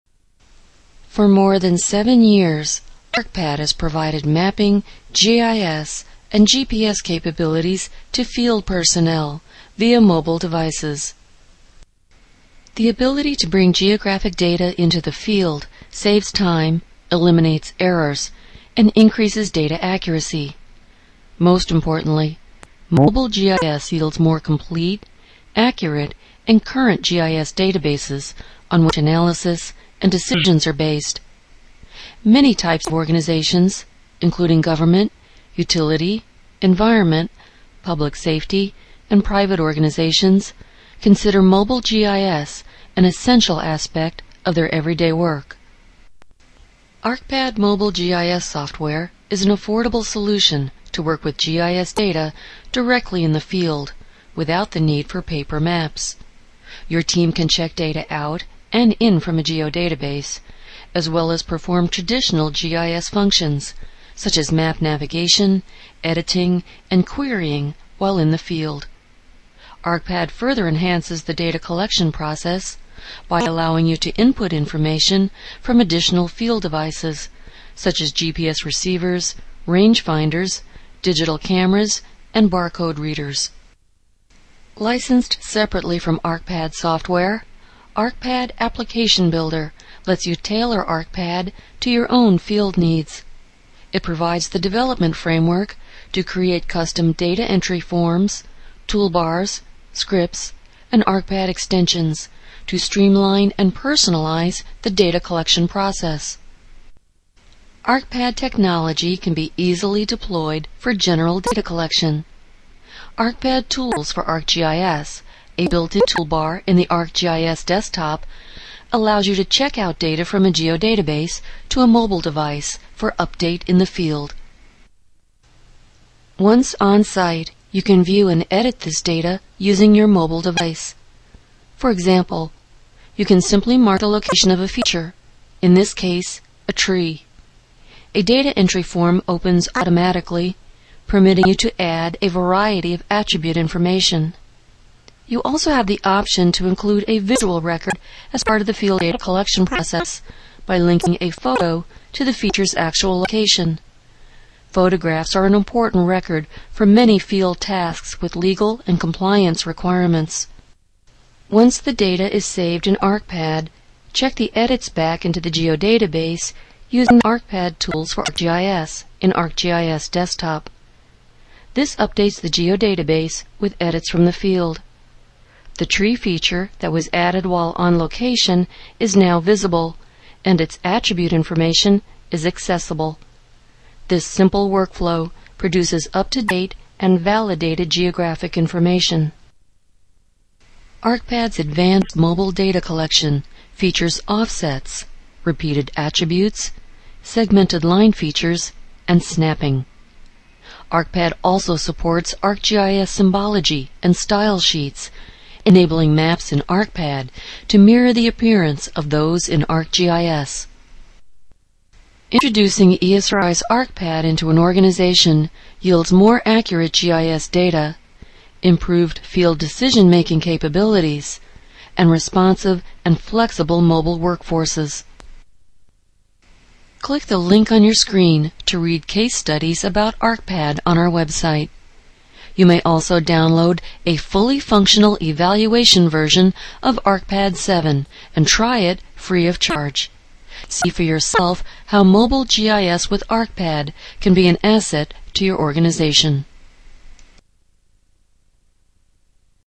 ArcPad Mobile GIS
See an overview of ArcPad capabilities and the benefits of mobile GIS.